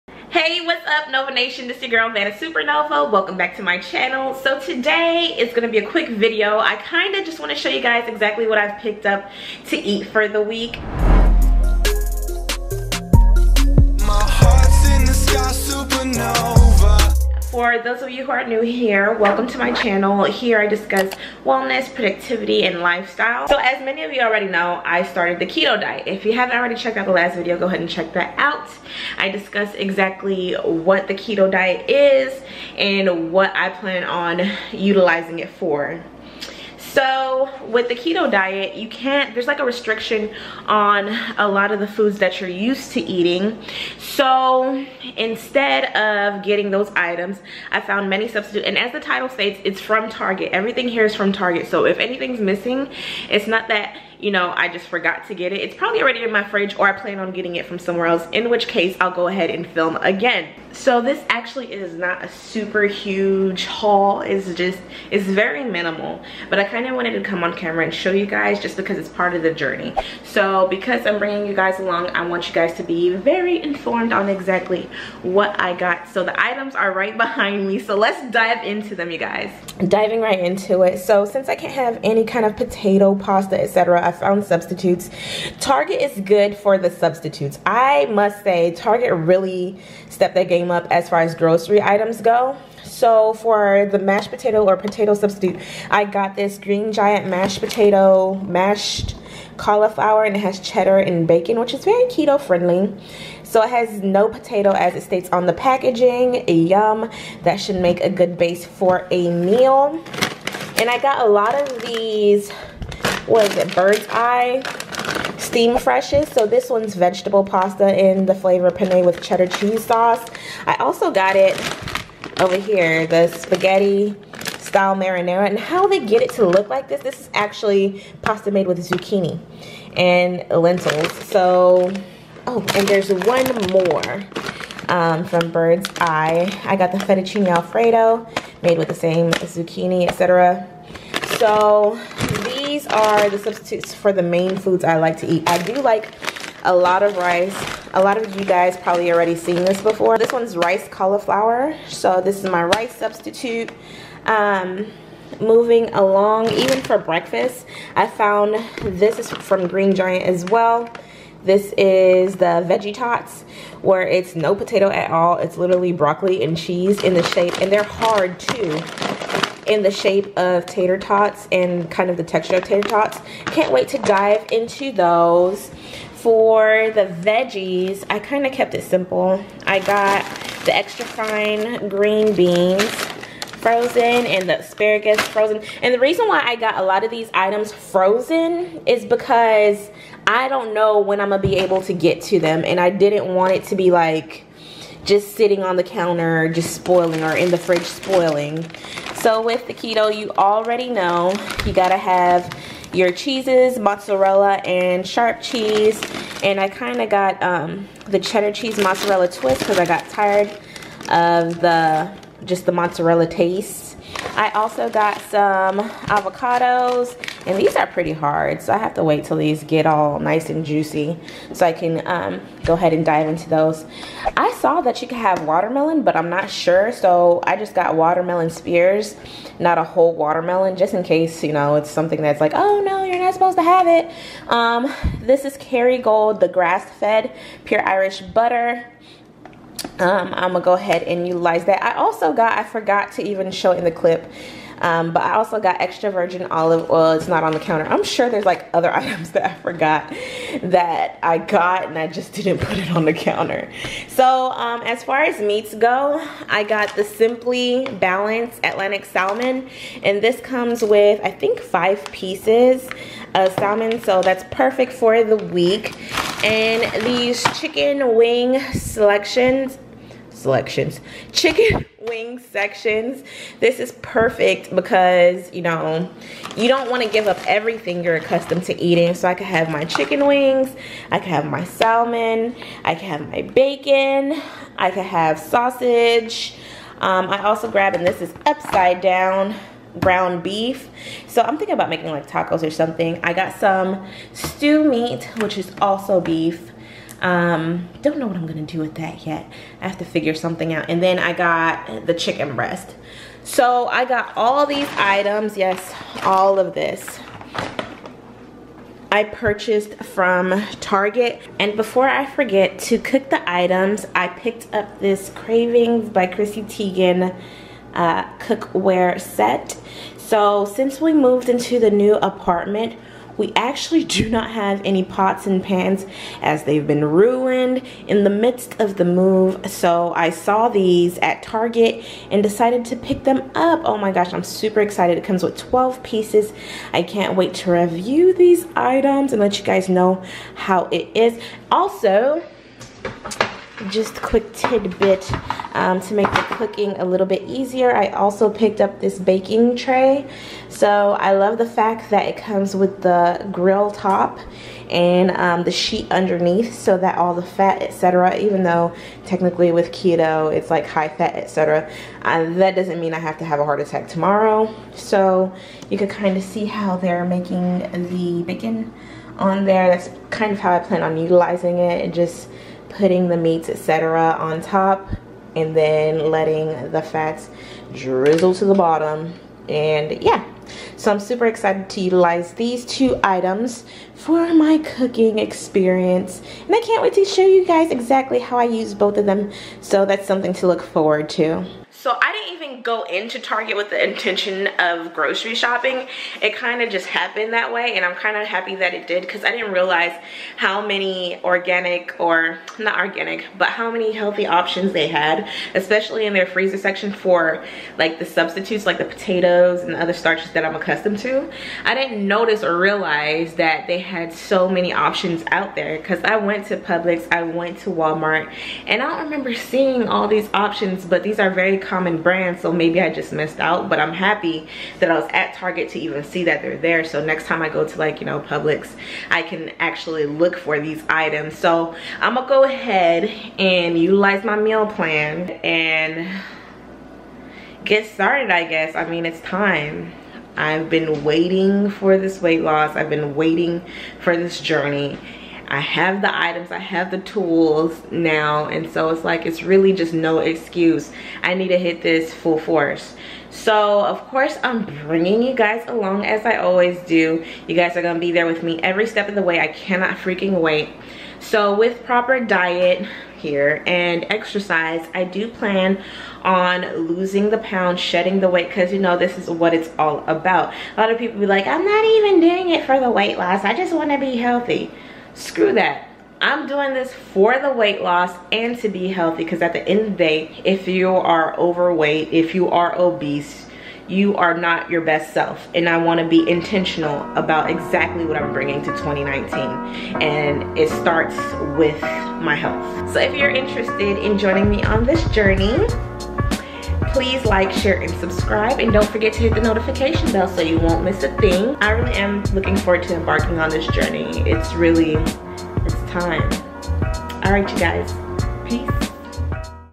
TARGET Keto Grocery Haul | My Keto Diet Journey
›»⟫ ♡ ! ! OPEN ME ! ! ♡ ⟪«‹

What's Up Nova Nation!

Today I am showing my TARGET Keto Grocery Haul . This is all apart of my Keto Diet Journey. I am still in the early stages of this journey so I am still learning as I go. Some items may not be considered Keto-friendly and I now know that. As I learn and grow I want to help you learn and grow as well.

► FAQ
- Camera: Canon SL2, Canon g7x mark ii, iPhone Xs
- Microphone: MOVO VXR10
- Tripod: Sirui 3T-35K